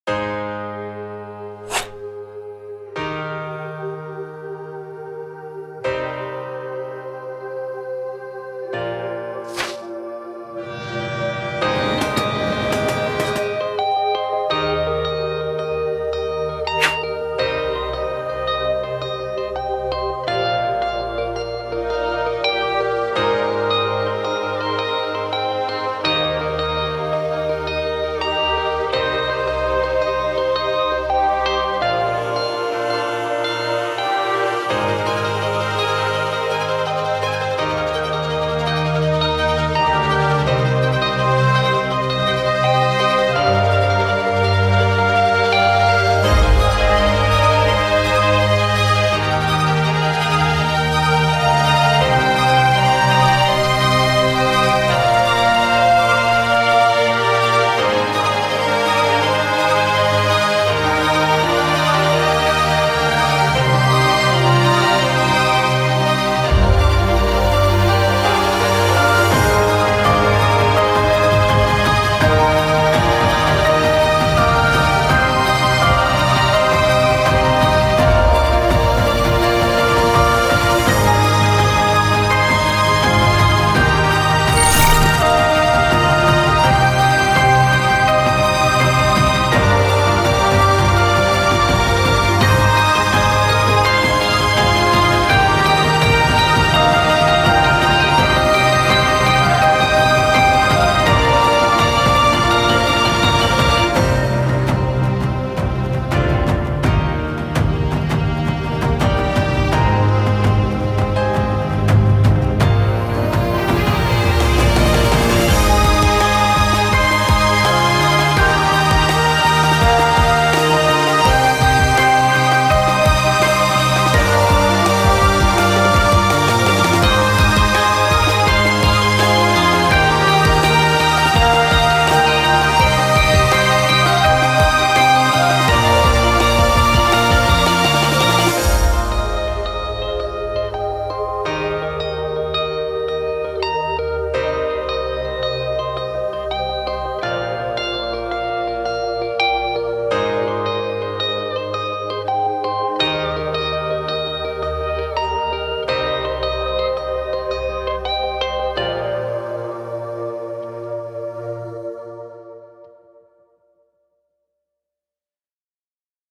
DHAKA CITY CINEMATIC | 4K DRONE NIGHT & DAY | TRAVEL BANGLADESH
Dhaka is a wonderful City. The City of Joy The city of Love. The city of Peace and a City which is livable.
The city which i have grown up it is simply amazing in this video you will be seeing the different parts of Dhaka city with amazing and wonderful view hope you guys will be liking it.

Item ID:                         H9E2CEH
Author Username:                 matsteiner-music
Licensee:                        Rakiur Rahman
Registered Project Name:         Skytography
License Date:                    April 18th, 2022